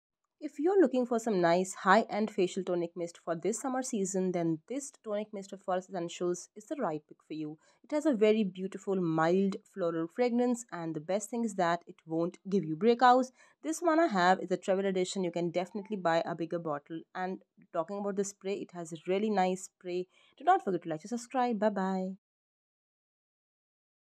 Best Toner for this summer Forest Essentials Tonic mist Review
This forest essentials panchpushp facial Rose tonic suits all skin types- oily, acne prone, dry and sensitive skin.

This toner is perfect for this summer season and will make your skin hydrated.